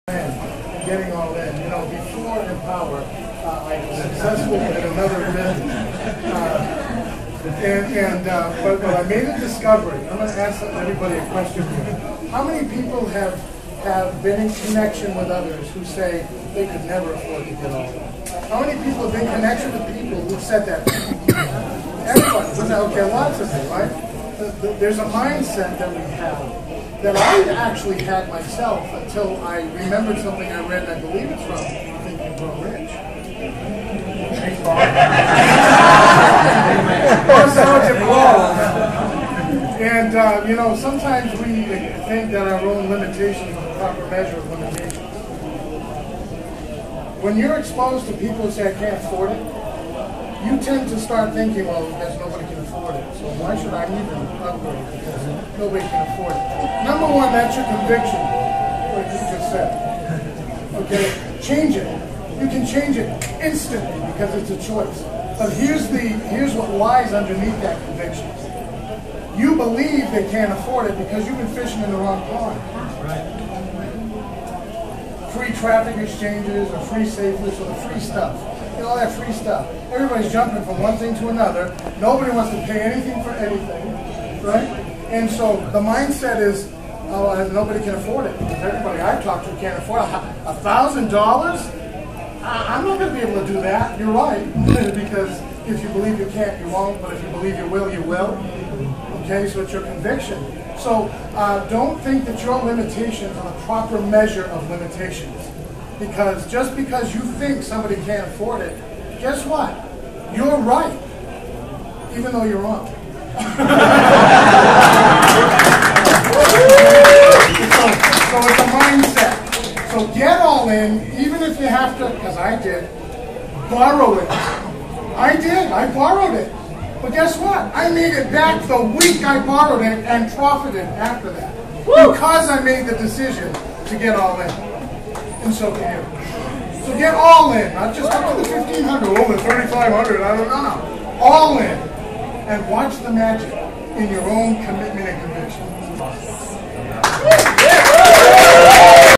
MLM Tips - Mindset - Set The Foundation, Believe
Kevin Sousa made a discovery. Who has heard someone say, "I don't have the money." It's a Mindset we all have. We can change our conviction.